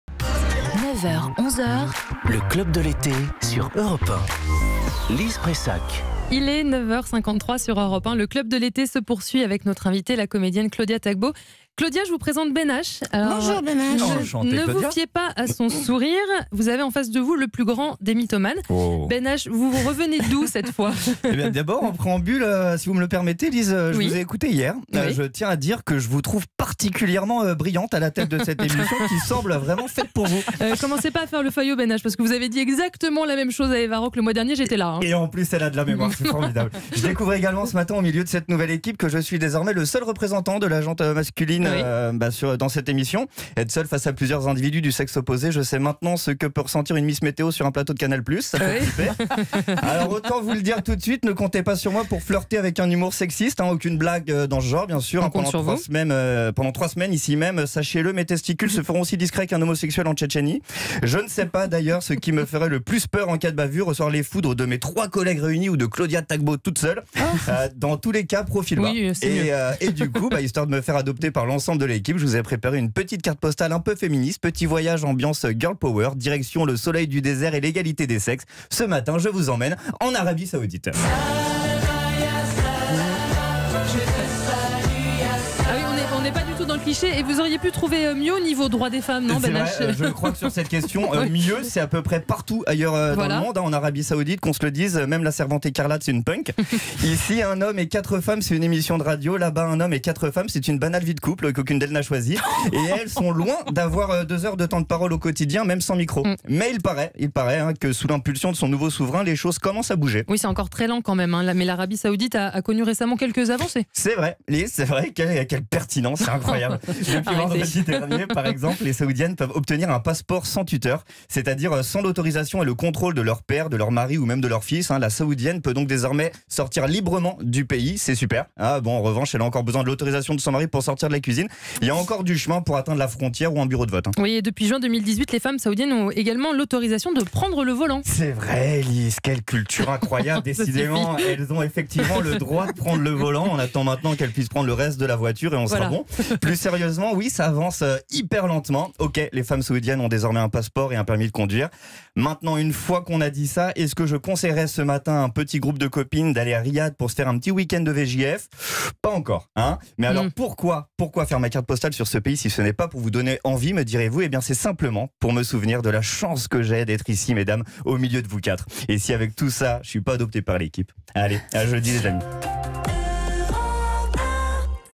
La carte postale mythomane de Ben H : l'Arabie Saoudite, le pays du soleil et de l'égalité des sexes
Tout l'été, Ben H nous dévoile avec humour ses cartes postales mythomanes.